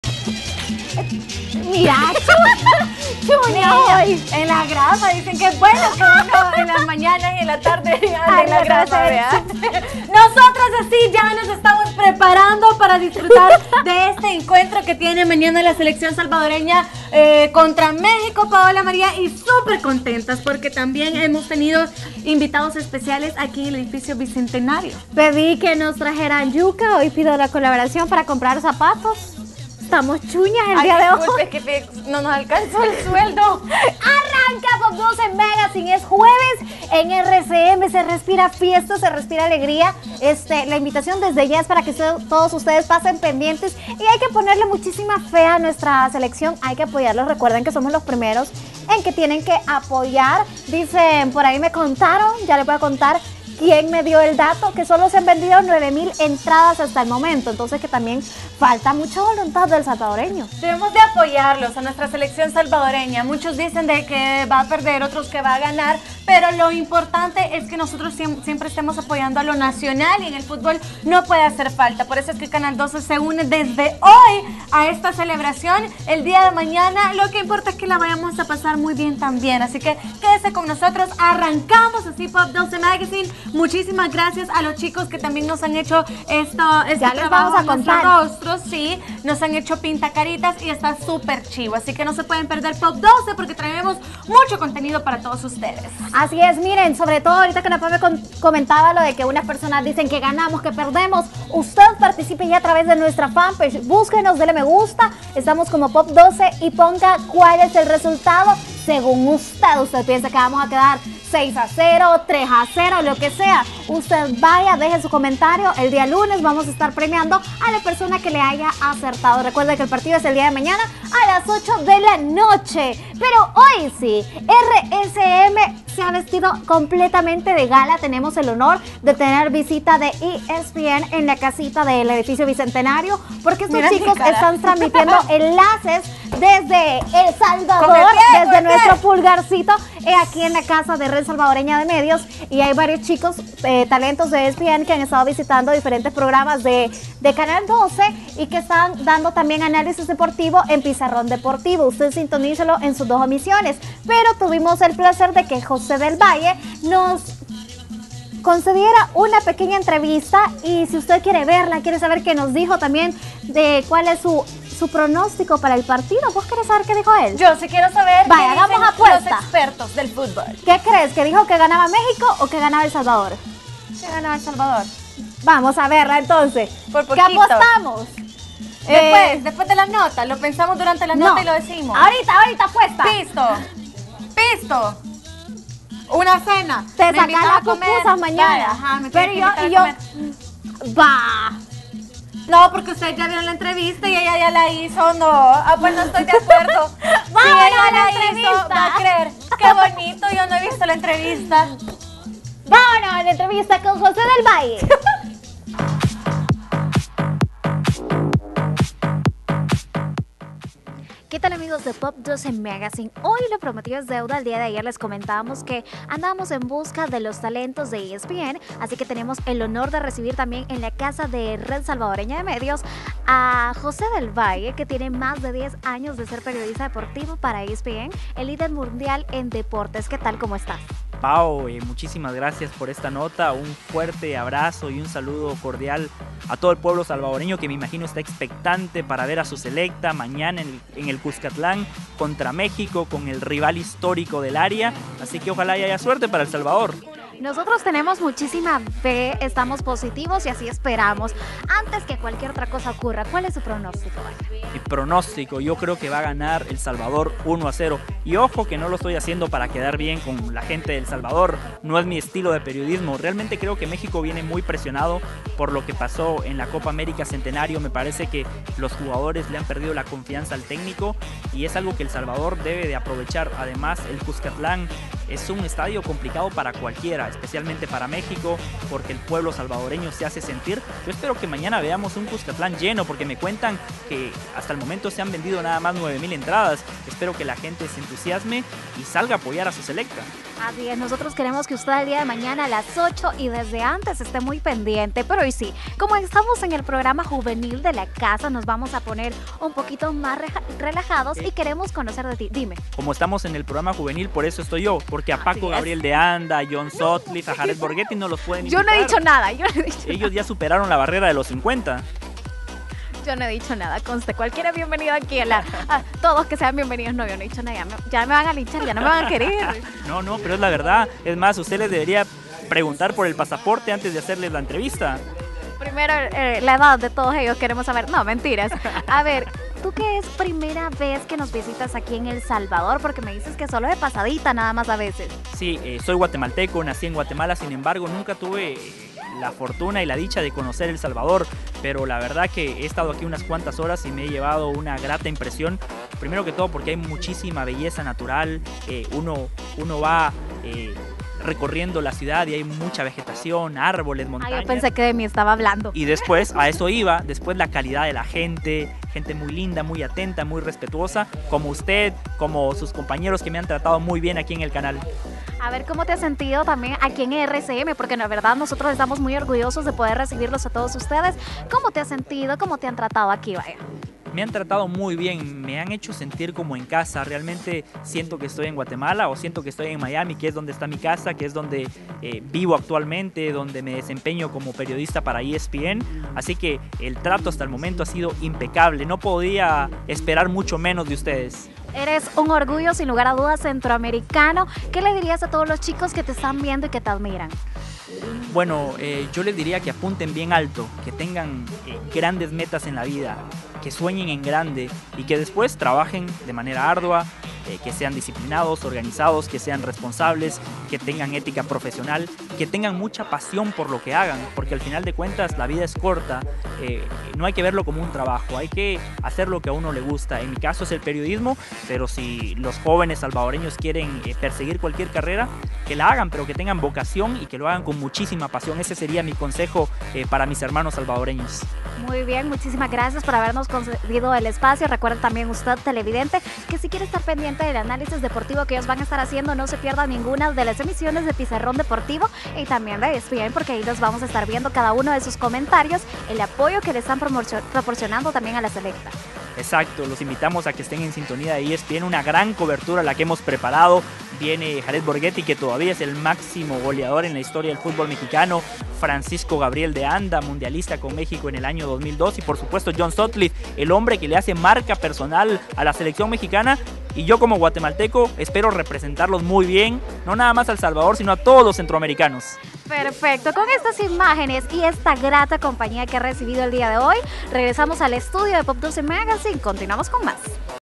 Pop 12 apoyando a la Selecta en la previa
En el set se respira fiesta de cara al duelo de mañana contra México.